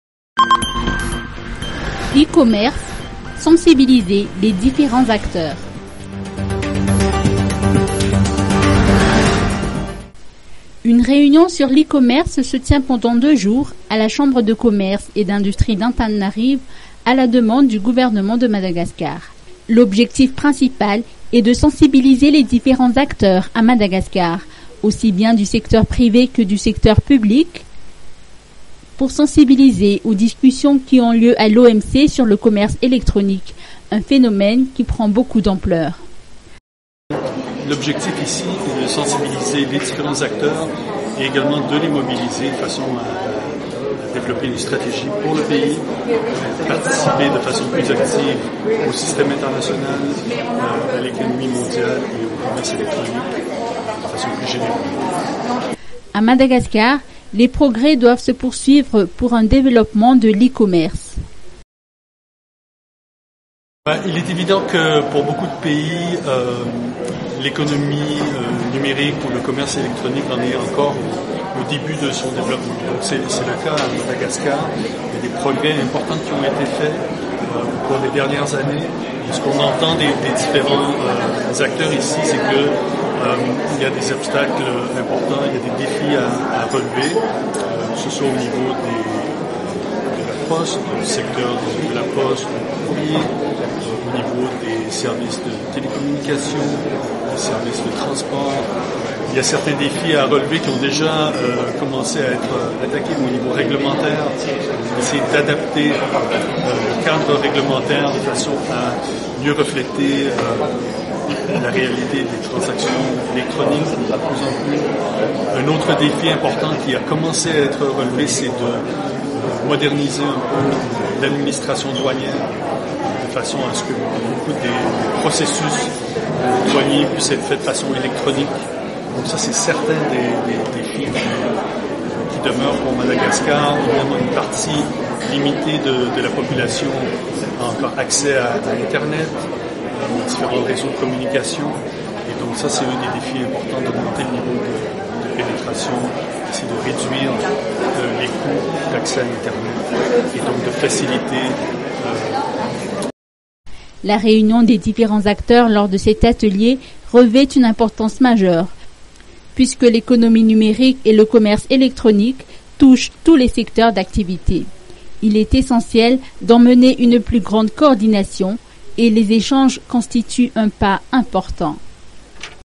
1 COMMERCE
Une réunion sur l'e-commerce se tient pendant deux jours à la Chambre de Commerce et d'Industrie d'Antananarivo à la demande du gouvernement de Madagascar.